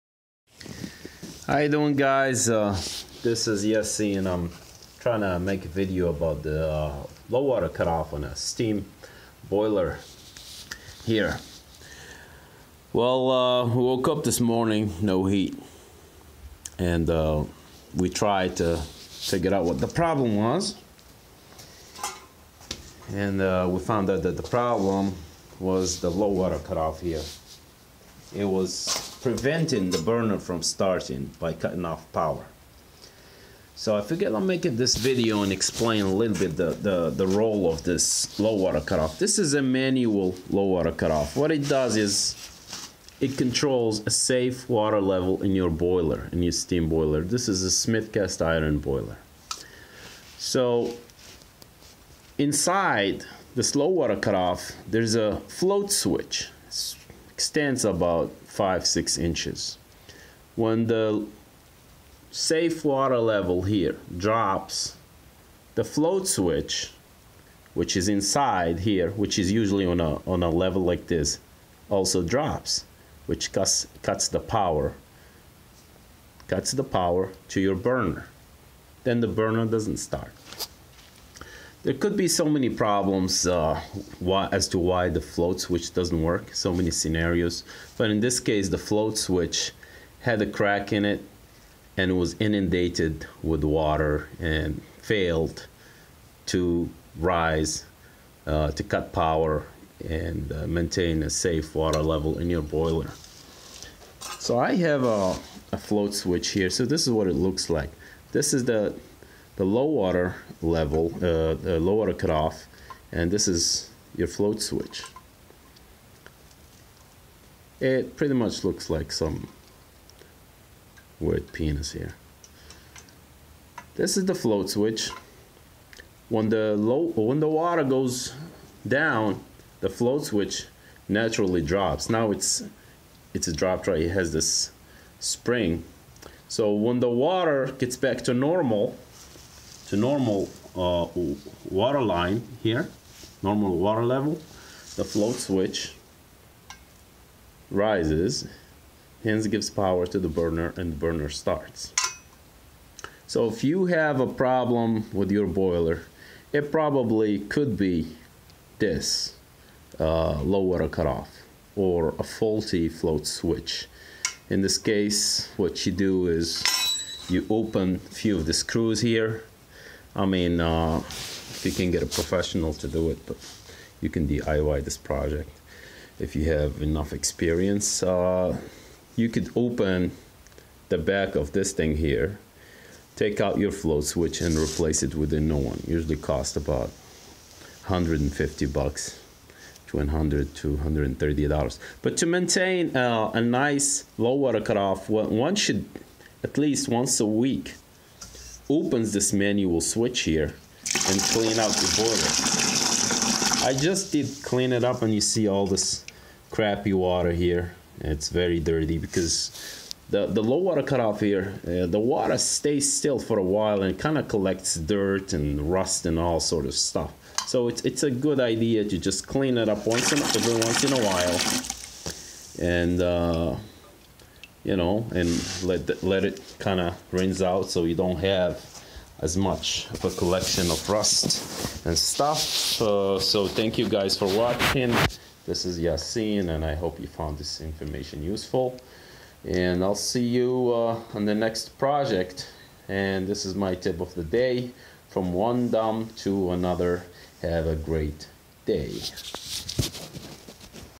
Low water cut off . prevents burner from starting
Fault Low water cut off . Prevents burner from starting on smith cast iron oil fired Boiler